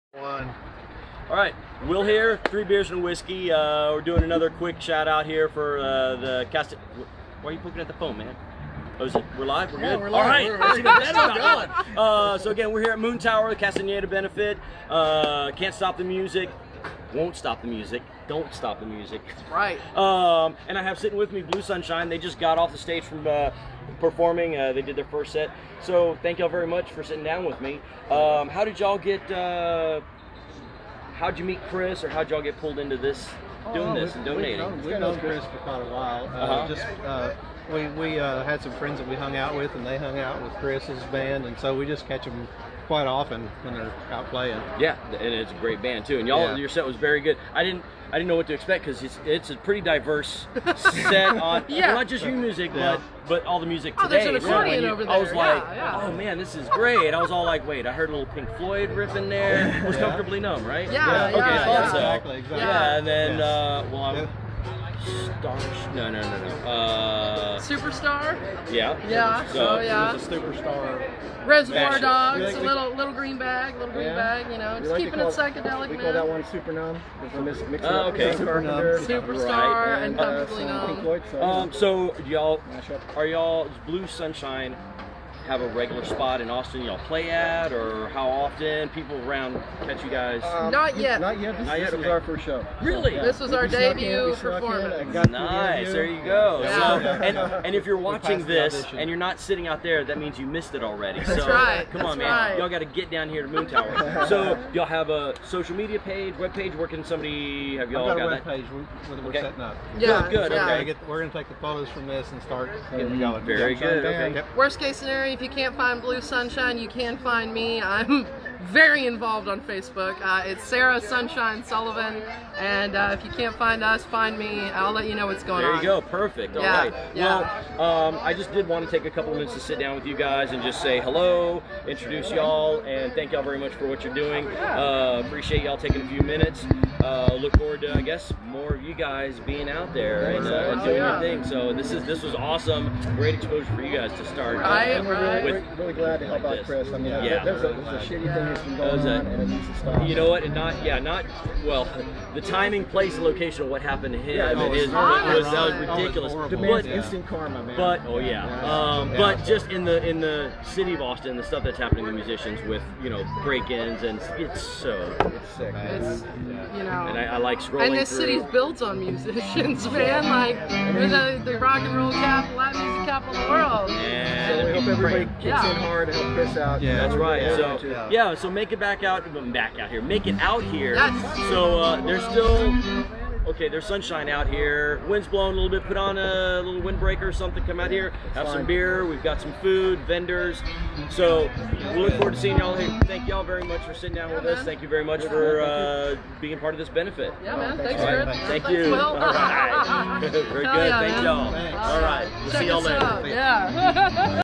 Three Beers and a Whiskey interview with Blue Sunshine
A talk show about anything that comes up. Interviews with musicians, bands, and the venue owners that support them.

Our first of a set of interviews that were done Live and on the Scene for the Castaneda Benefit. You will notice right away the questions are all pretty similar. But I do get to let the performer plug themselves and give a quick story about Chris Castaneda.
It was great fun and a great honor to have been part of this event.

Fun meeting and interviewing Blue Sunshine after their first ever live gig.